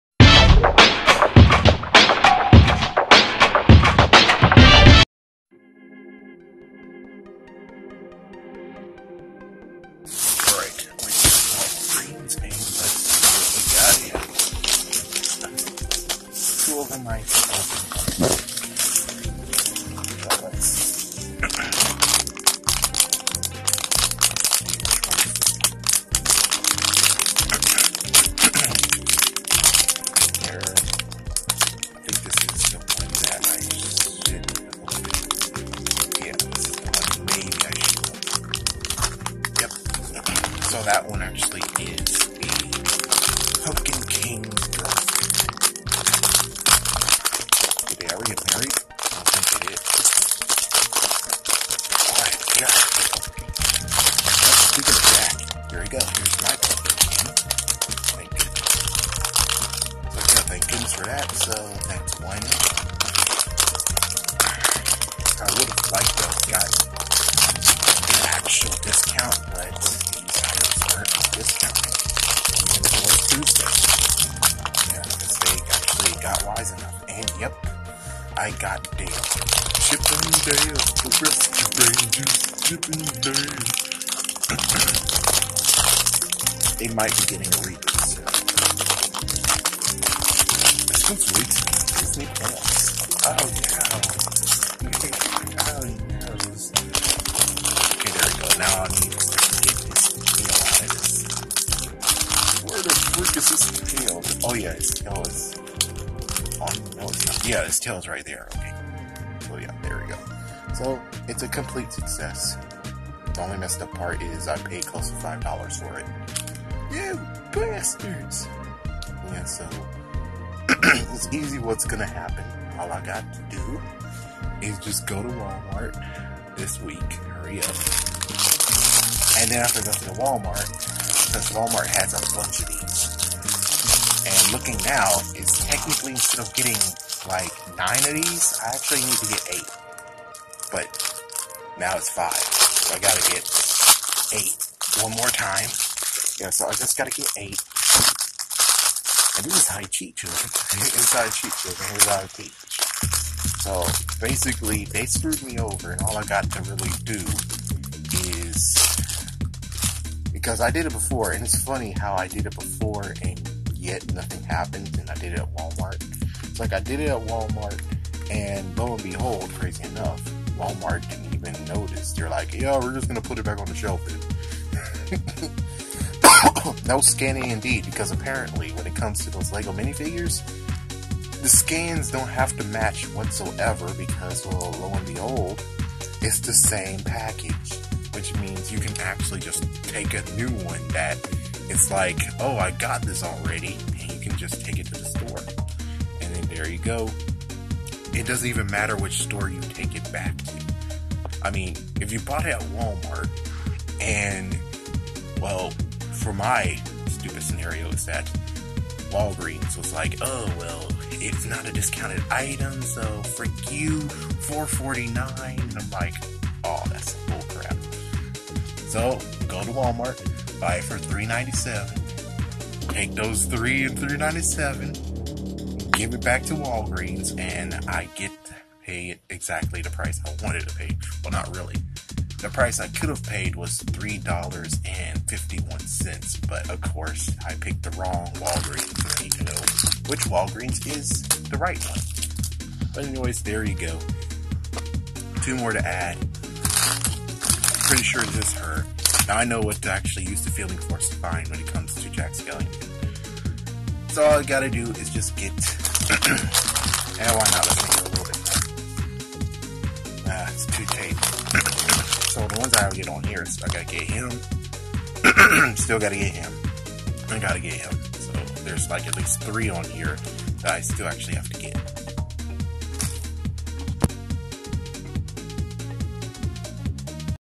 Deal Browsing @ Walgreens #9: Coming Soon VENGEANCE!!!!
Went to Walgreens on the special 15% day and. like usual, I chose the wrong one. The special price TAKEN from me! There will be vengeance!

Note: Vengeance does not include violence, arson, or sending Yosemite Sam to fix their wagon.